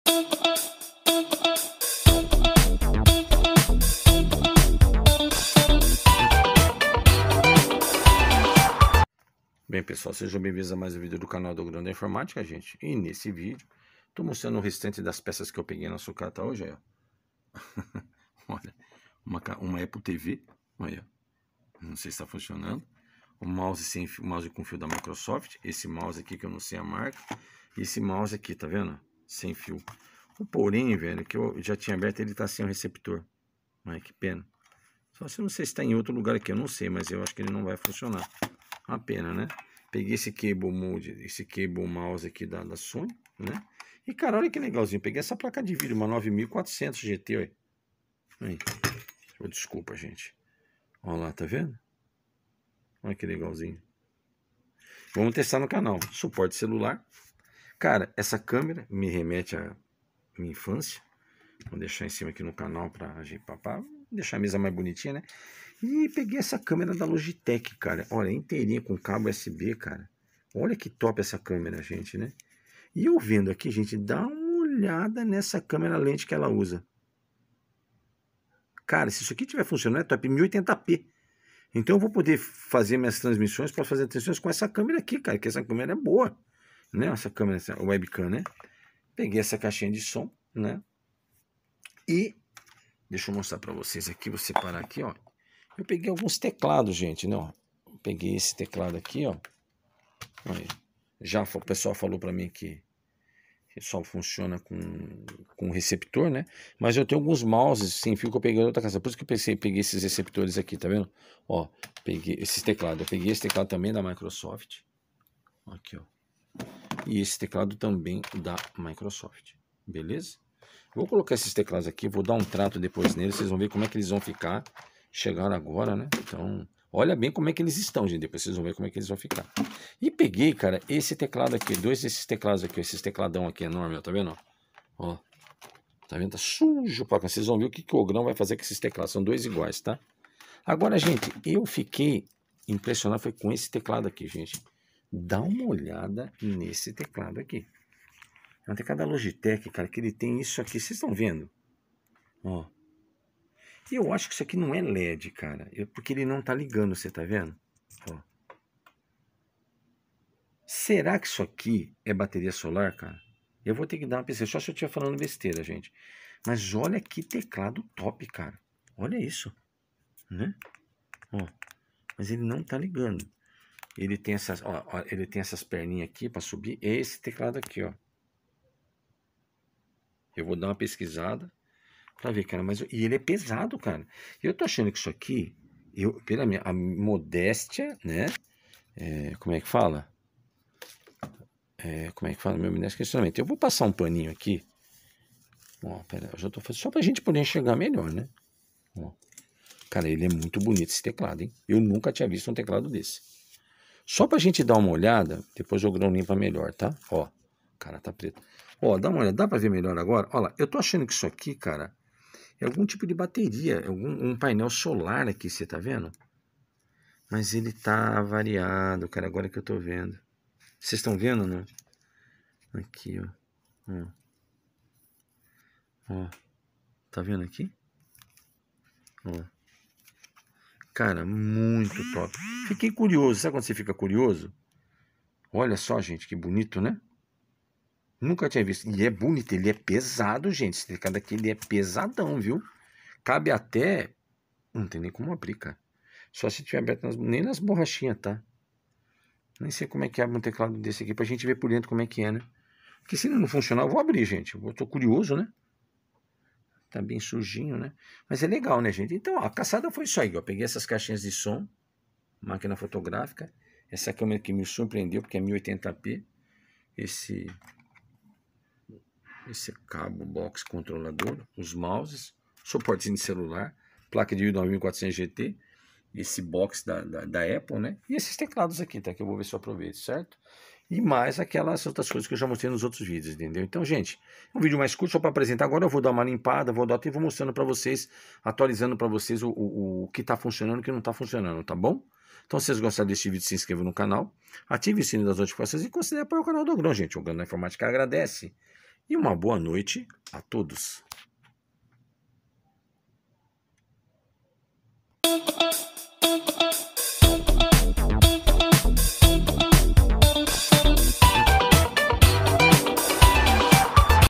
CAÇADA NO FERRO VELHO 29!!! MONTÃO DE COISA BOA!!!🤩🤩🤩
Nesse vídeo mostro o resultado da caçada no ferro velho 29!!! Confira!!! Assista!!! Apoiar o canal Pix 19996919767. Me siga no Instagram : luizperez67_